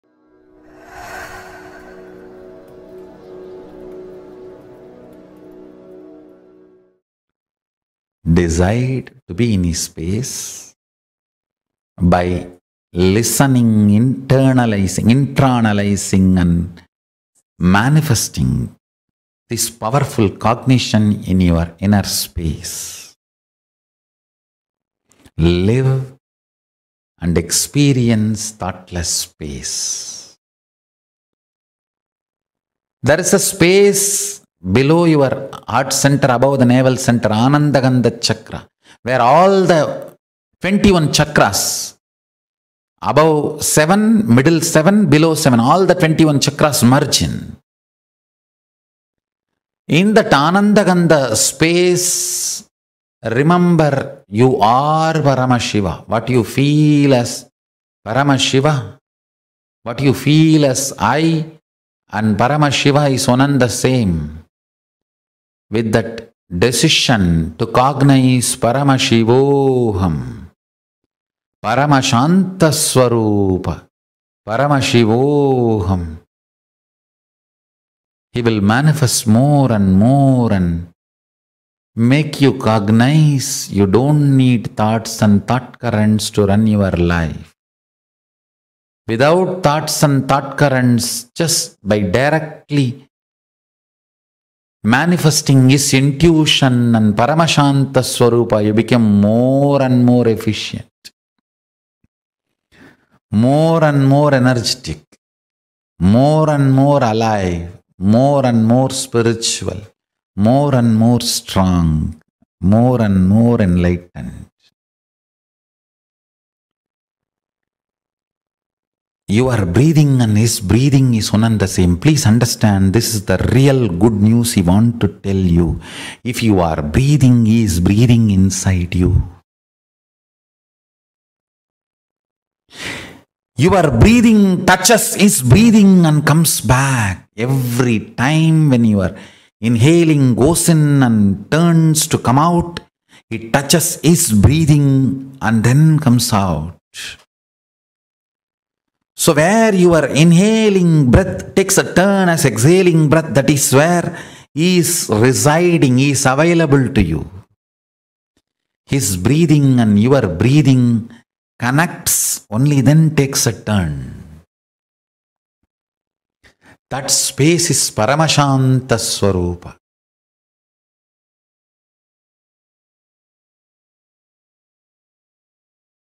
Your Breath Reaches Paramashiva and Comes Back | Inhale Exhale Nithyananda Kailasa
Decide to be in a space by listening, internalizing, intra analyzing and manifesting this powerful cognition in your inner space. Live and experience thoughtless space. There is a space below your heart center, about the navel center Ananda ghanda chakra, where all the 21 chakras above seven, middle seven, below seven, all the 21 chakras merge in. In that Ananda Ghanda space remember you are Paramashiva, what you feel as Paramashiva, what you feel as I and Paramashiva is one and the same. With that decision to cognize Paramashivoham, Parama Shanta Swarupa, Paramashivoham. You will manifest more and more , and make you cognize you don’t need thoughts and thought currents to run your life. Without thoughts and thought currents, just by directly manifesting this intuition and Parama Shanta Swarupa, you become more and more efficient. More and more energetic, More and more alive. More and more spiritual. More and more strong. More and more enlightened.Your breathing and His breathing is one and the same. Please understand, this is the real good news He wants to tell you. If you are breathing His breathing inside you, your breathing patches His breathing and comes back. Every time when you are inhaling, goes in and turns to come out, it touches His breathing, and then comes out. So, where you are inhaling, breath takes a turn, as exhaling breath that is where He is residing, He is available to you. His breathing and your breathing connects, only then takes a turn. That space is Parama Shanta Swarupa.